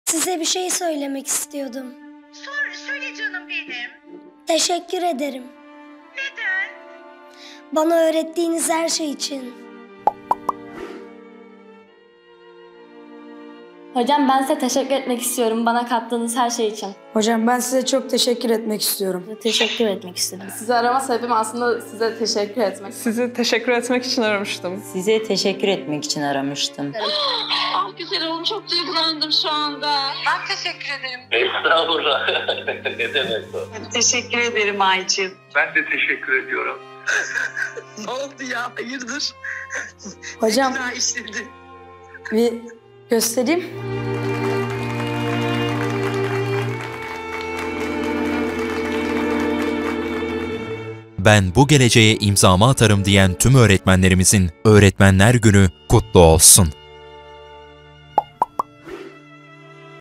Öğretmenlerimizi Arayıp Sebepsizce Teşekkür Ettik ❤️ Duygusal tepkiler
Bahçeşehir Kolejinin öğrenci ve mezunlarından geleceğe imza atan öğretmenlerine duygusal mesajlar var ❤️